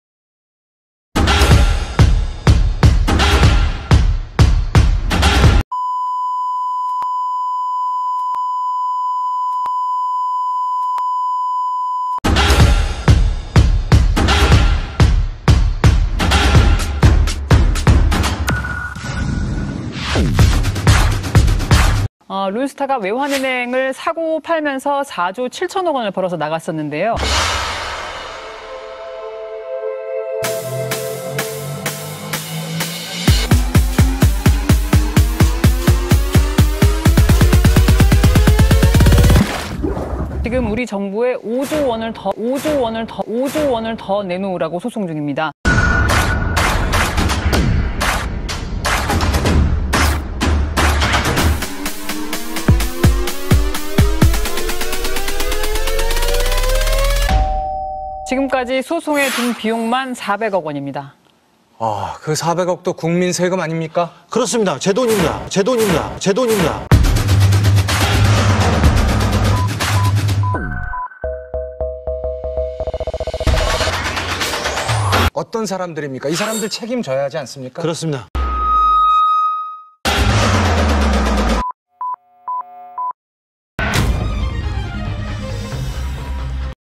[엠빅비디오] 9만 원이 아니고 29만 원이었다는
9만 원이 끝인 줄 알았죠?
하베스트 4조 5천억 론스타 4조 7천억 그리고 소송 5조 원까지
도합 12조 2천억을 대한민국 국민 인구로 나누면?